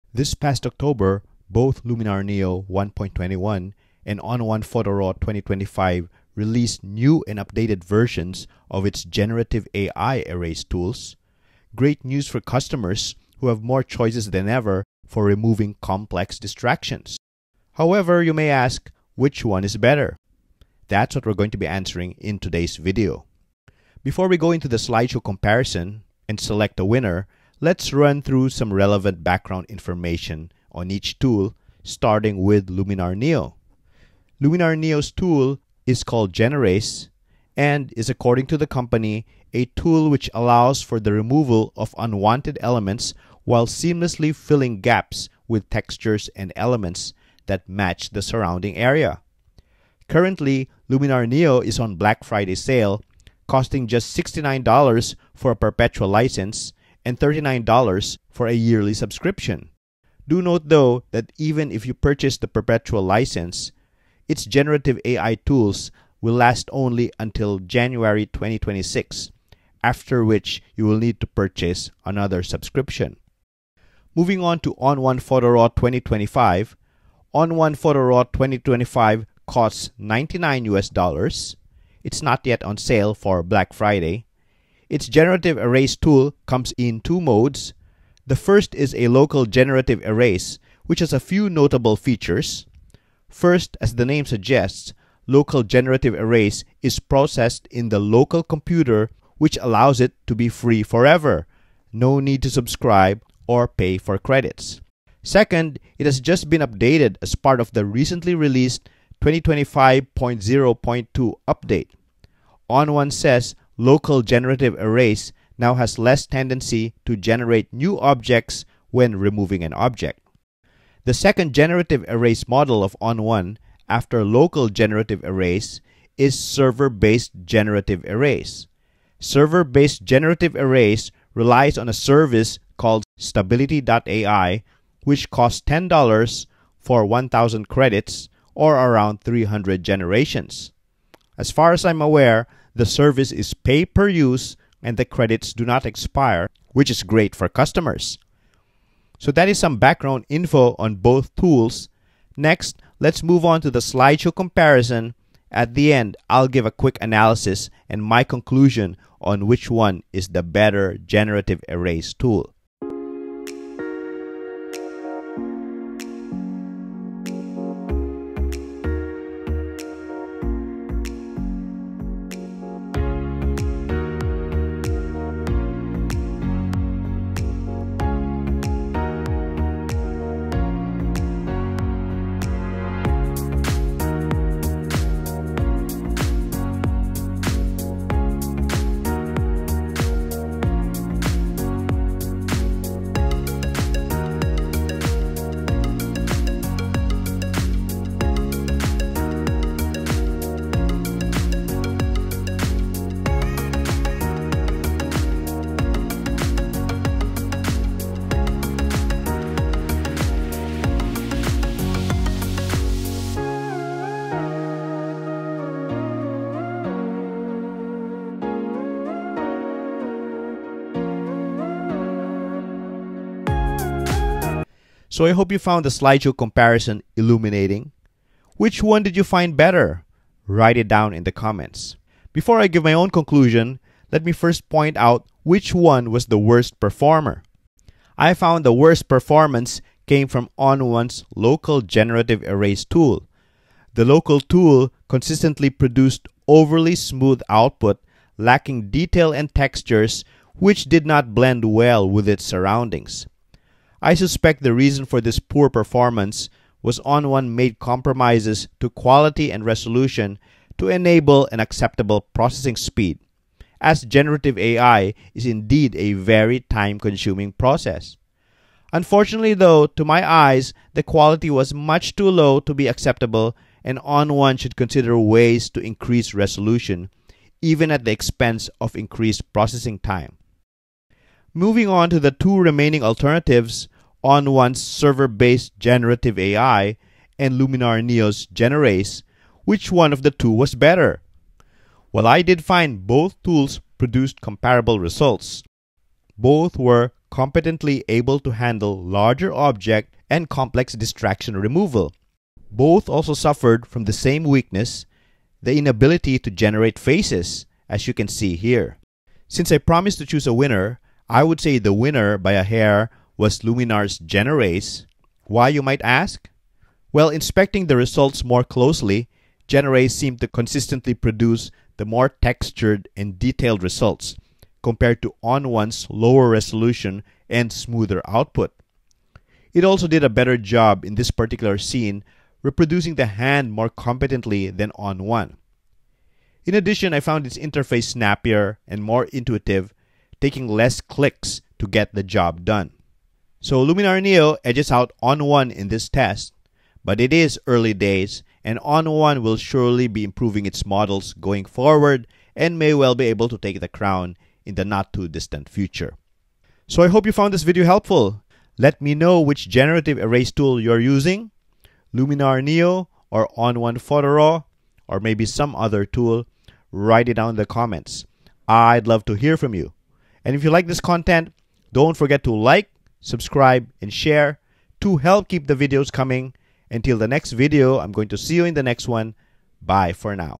ON1 PHOTO RAW 2025 VS LUMINAR NEO 1.21. WHICH HAS THE BETTER GENERATIVE ERASE?
This past October both Luminar and ON1 released updated or new versions of powerful generative erase tools. In this video we answer the question which one is better.